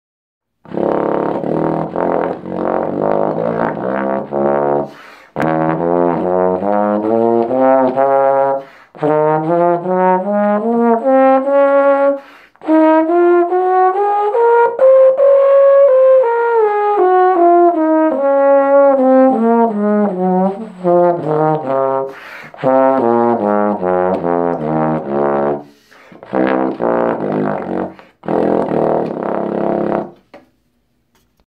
Db Major - Bass Trombone
4 Octaves

Db Eb F Gb Ab Bb C Db

Trombone: Conn 62HI
Mouthpiece: Conn 1.5G